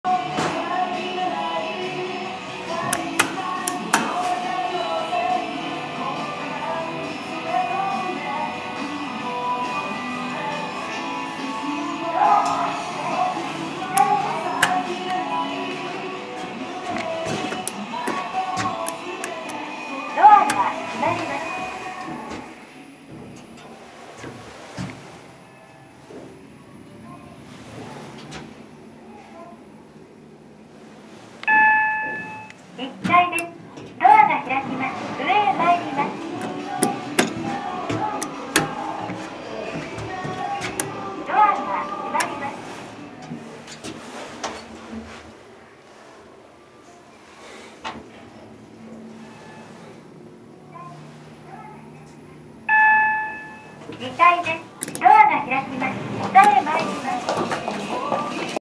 【解体に伴い現存せず】卸売スーパー美しが丘店のエレベーター
メーカーはオーチス製です。Gen2ですがアナウンスが古めです。

用途：乗用
定員：15名
積載：1000kg
設置年：2011年
アナウンス：あり
到着音：あり
車椅子操作盤：あり
エレベーター台数：2
機種：Gen2
方式：ロープ式？